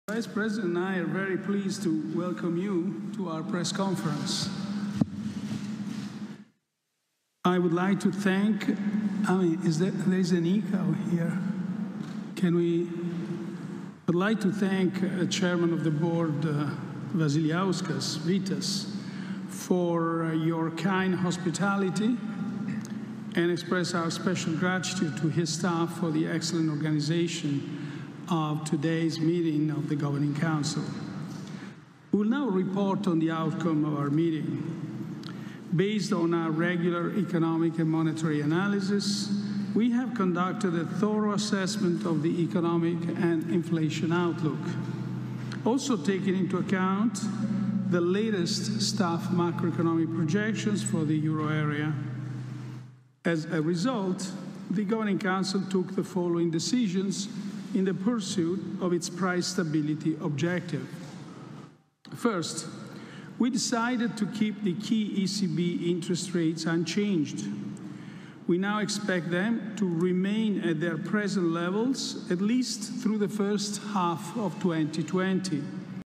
Draghi: "I tassi di interesse in Europa invariati almeno fino a metà 2020"
(Agenzia Vista) - Francoforte, 6 Giugno 2019 - "Il Consiglio Direttivo della BCE si attende che i tassi di riferimento si mantengano su livelli pari a quelli attuali almeno fino alla prima metà del 2020". Lo ha annunciato il Presidente Mario Draghi in conferenza stampa. Courtesy YouTube BCE